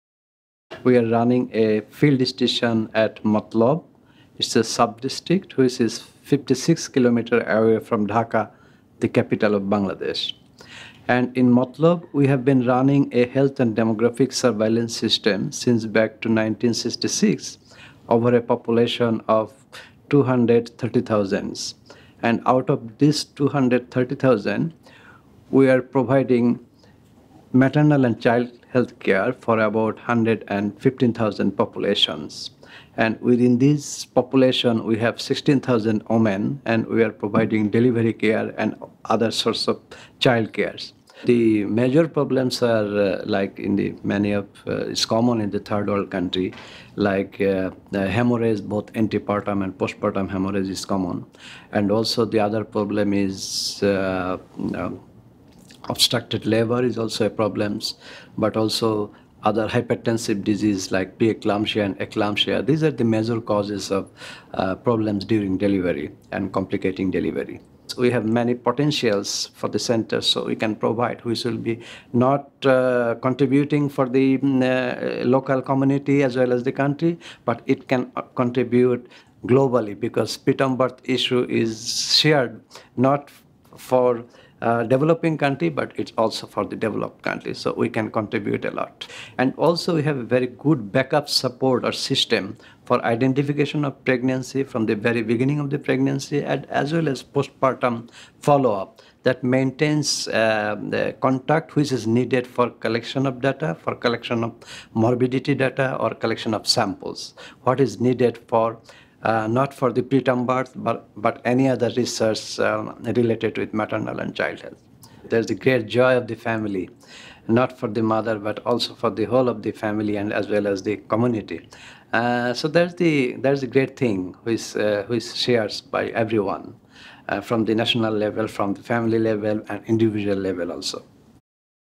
Dr. Anisur Rahman - Preventing Preterm Birth initiative
New innovative research sites aiming to study premature birth have been announced by the Global Alliance to Prevent Prematurity and Stillbirth (GAPPS), an initiative of Seattle Children's.

The sites are funded through the Preventing Preterm Birth initiative, part of the Bill & Melinda Gates Foundation's Grand Challenges in Global Health. The initiative seeks to discover biological mechanisms that lead to preterm birth and develop innovative strategies for prevention, with particular focus on solutions relevant to low- and middle-income countries, where 99% of the world's infant deaths occur.